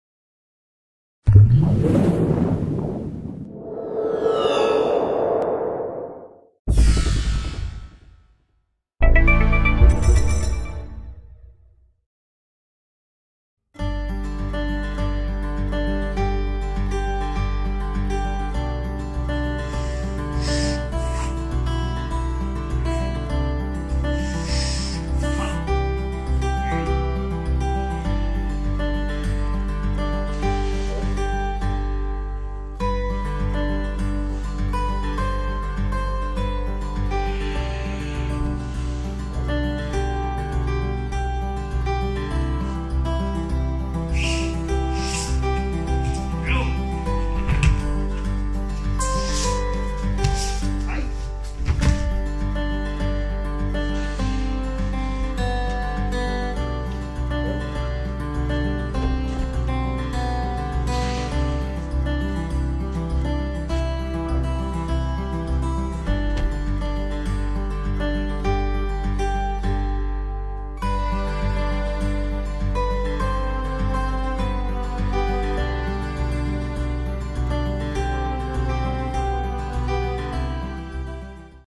Katatedori IRIMINAGE (jodan) - Marco Rubatto
Forma di base (kihon) e fluida (ki no nagare) di "presa al polso e proiezione con il corpo che entra" (katatedori iriminage), forma alta (jodan)